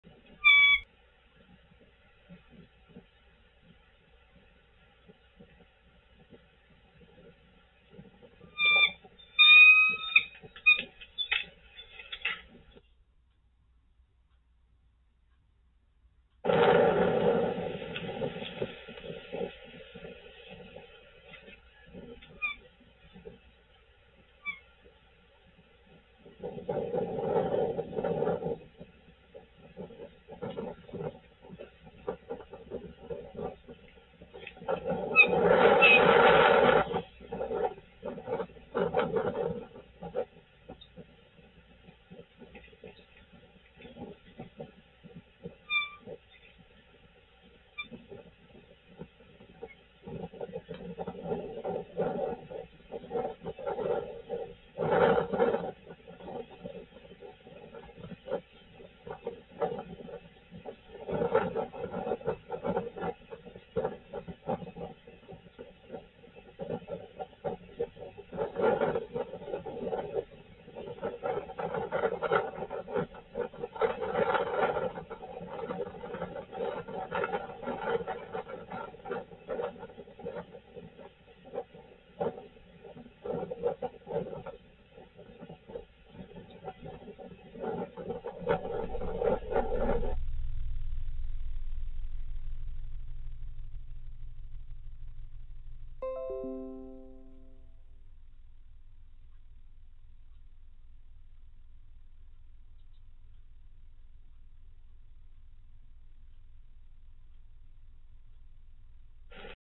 DH2019, Obey With Stick, Hits River, 021319
Obey had just been relieved of incubating duty and immediately returned with a big stick. Appeared to step on River and bonk her with it.
This video was recorded with permission of Dale Hollow Eagle Cam, Dale Hollow Marina Assoc., and TwinLakes Telephone Coop.